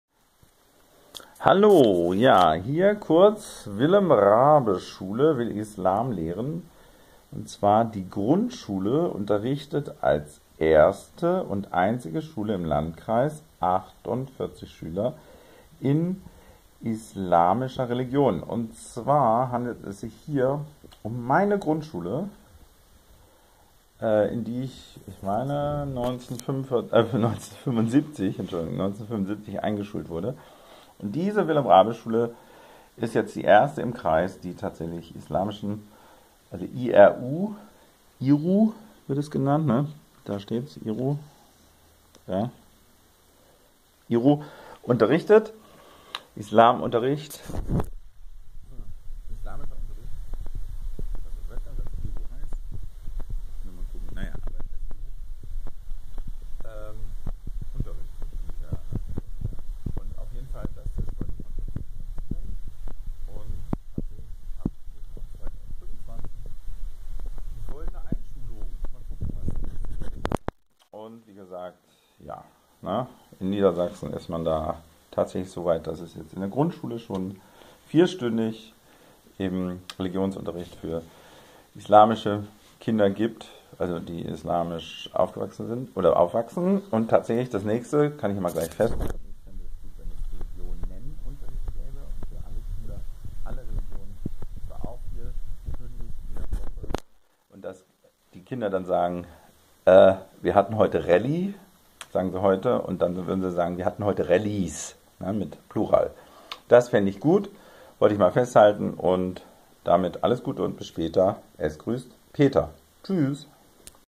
IRU an der GS Wilhelm Raabe Schule in WF
Islamischer ReligionsUnterricht gibt es jetzt in Niedersachsen an ca 60 Schulen, auch an der Grundschule Wilhelm Raabe Schule in Wolfenbüttel, in die ich 1975 eingeschult wurde. Meine Idee: ReligionEN Unterricht für alle SchülerInnen, also: alle Religionen für alle SchülerInnen. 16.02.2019